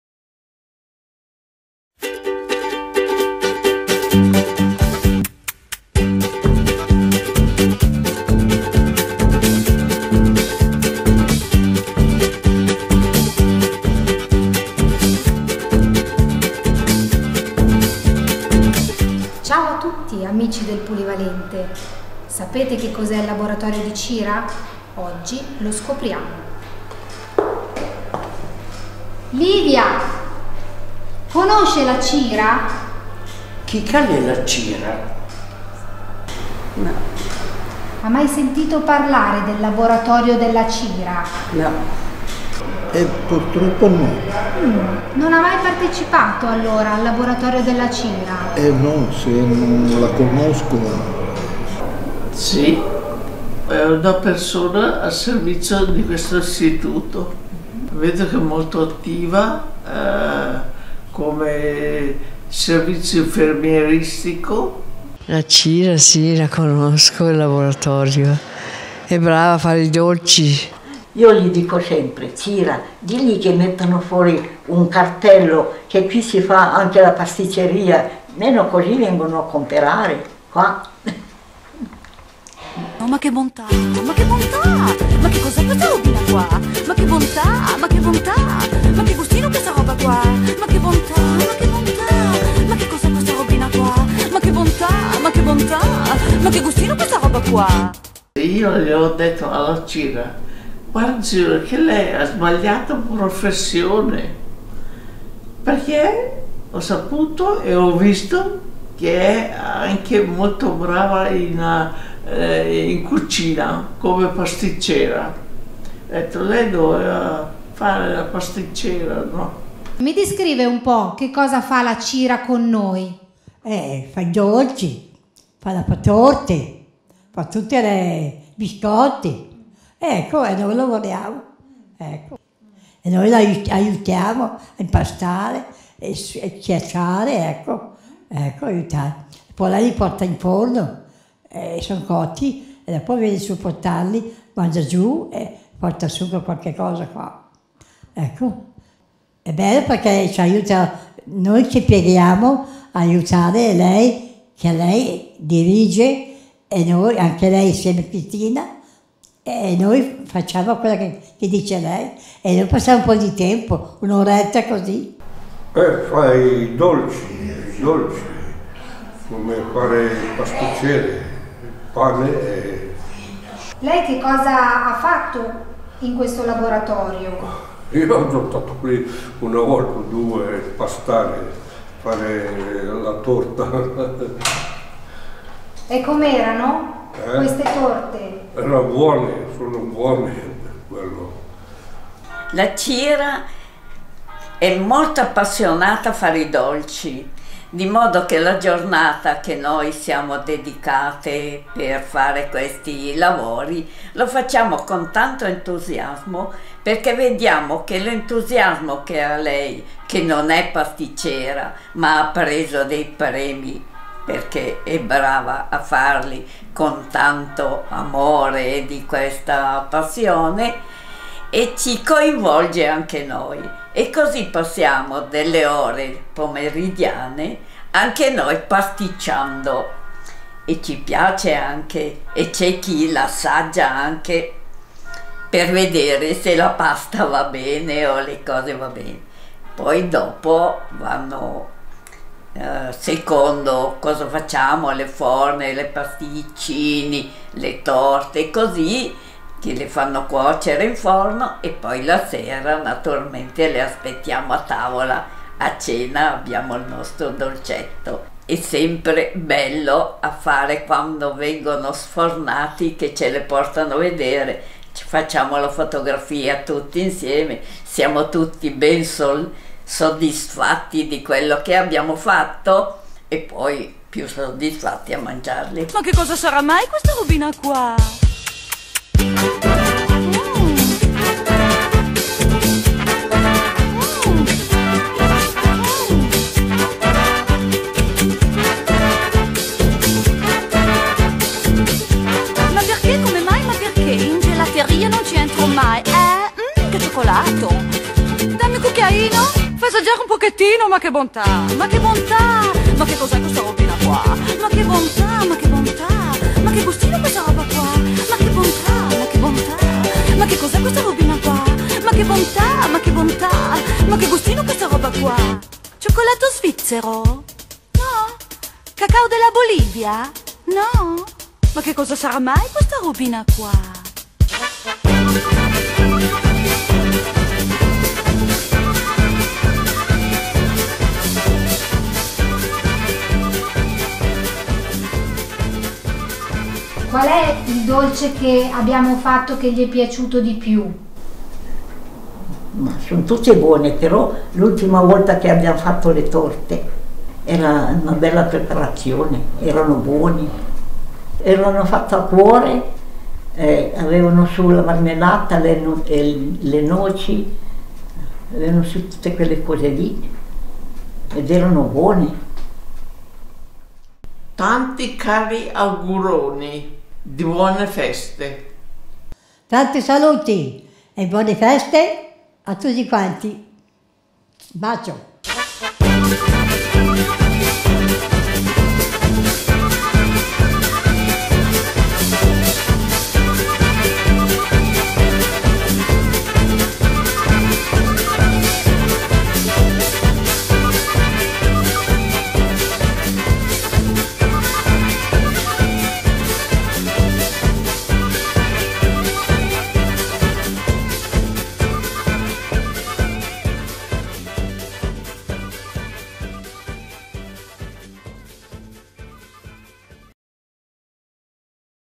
Il Calendario 2017 dell'Asfarm di Induno Olona
Presentato il Calendario 2017 dell'Asfarm di Induno Olona, dedicato al "Laboratorio di Cira", una delle attività di animazione del Centro Polivalente Anziani, realizzato con la collaborazione di Roberto Rinaldini.